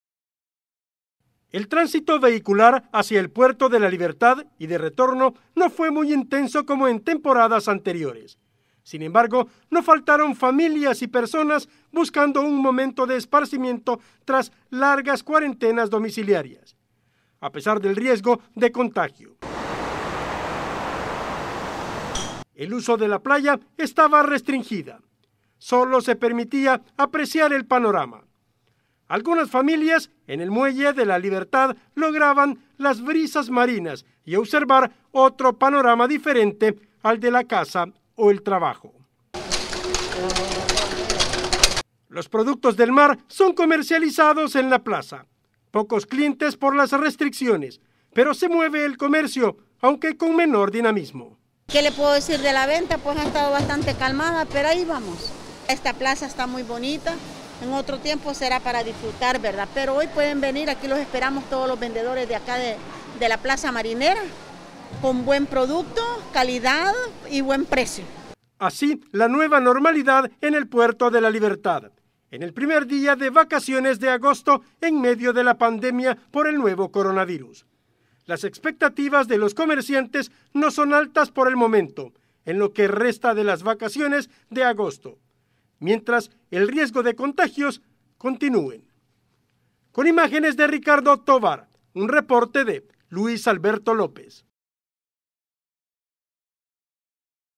Afluencia de turistas en El Puerto de La Libertad de El Salvador en tiempos de pandemia
La Organización Mundial de Turismo (UNWTO) calcula las pérdidas en el sector turismo de hasta el 80%.

¡Visítenos!